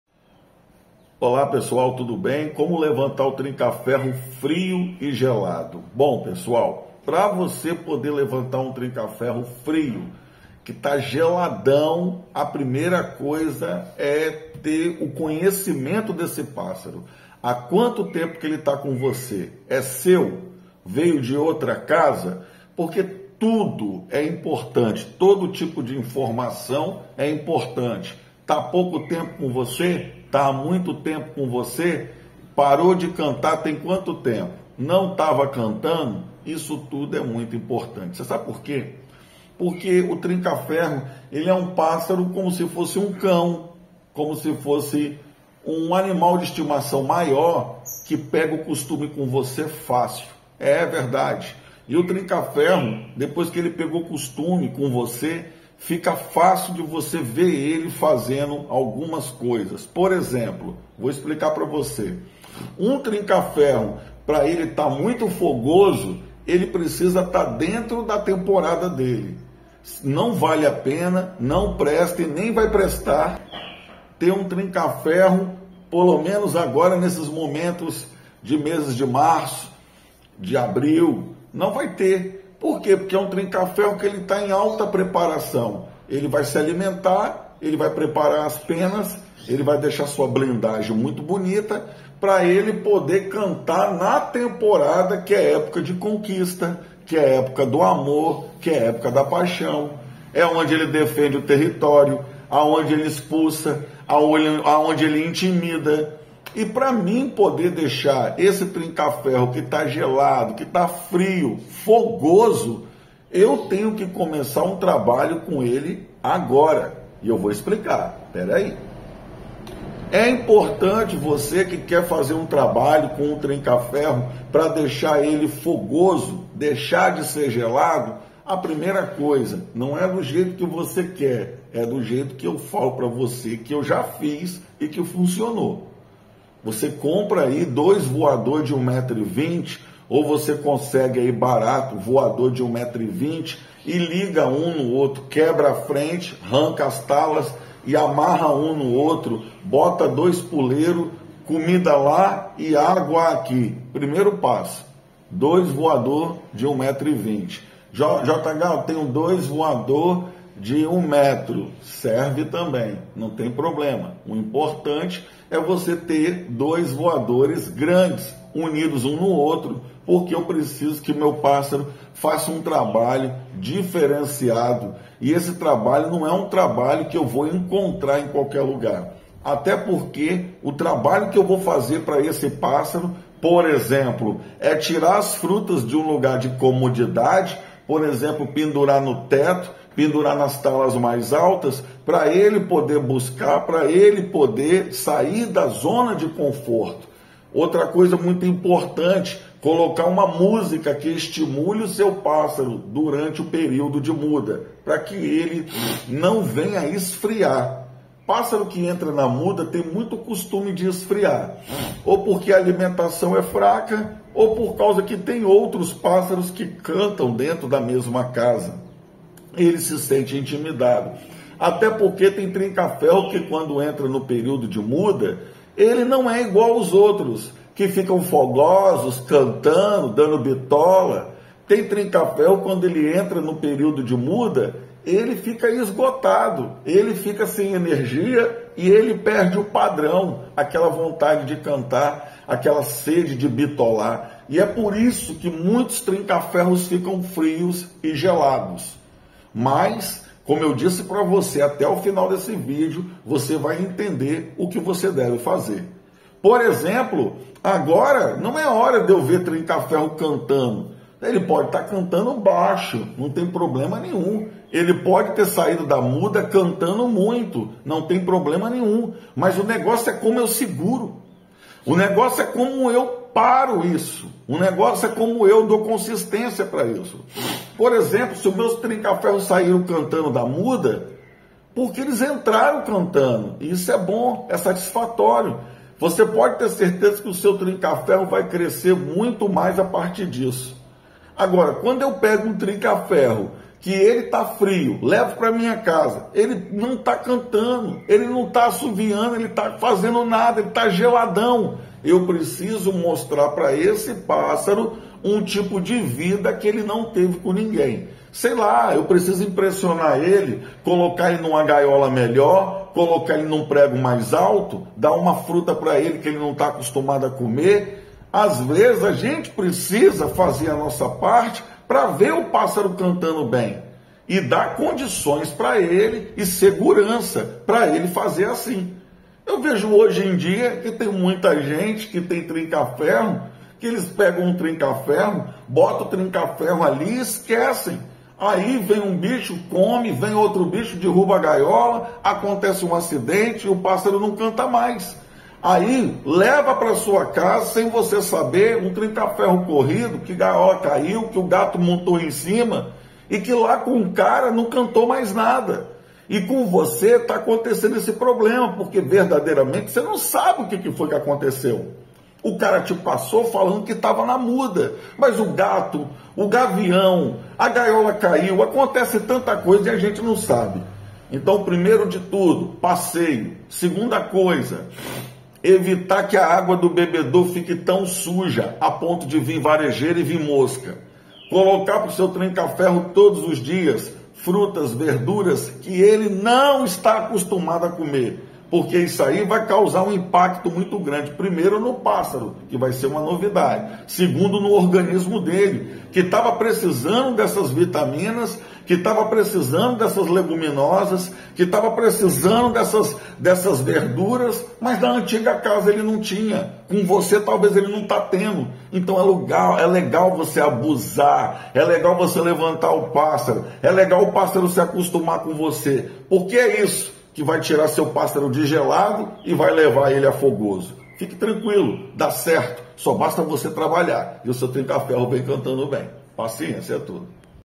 MEXIDA FÁCIL LEVANTAR TRINCA-FERRO FRIO GELADO .
fala rapaziada fala Brasil como levantar trinca-ferro frio gelado é esse é o tema do nosso vídeo que vamos assistir espero que vocês gostem É isso mesmo vamos ensinar um passo a passo como recuperar esse pássaro que ele tá tão bonito e falta cantar.